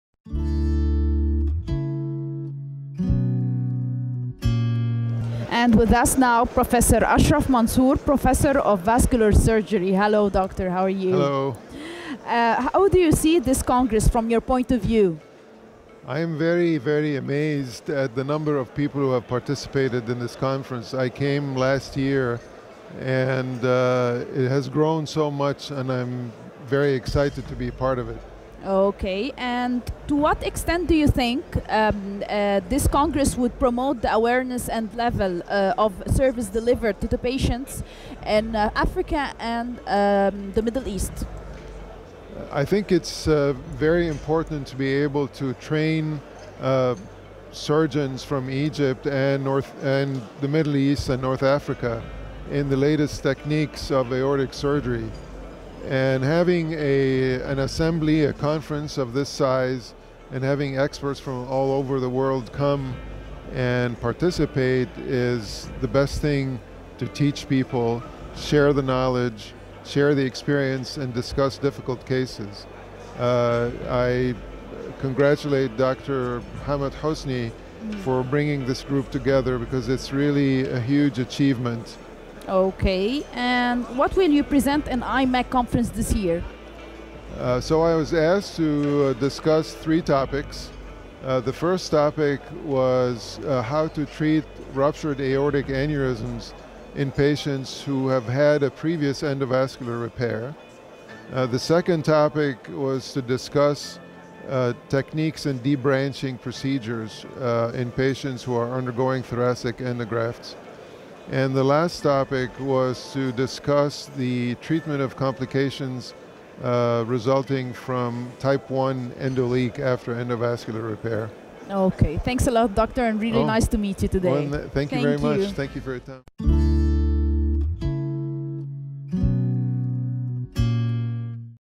Prof.Ashraf Mansour Talks about IMAC conference .
Professor of Vascular surgery .
أستاذ جراحة الأوعية الدموية.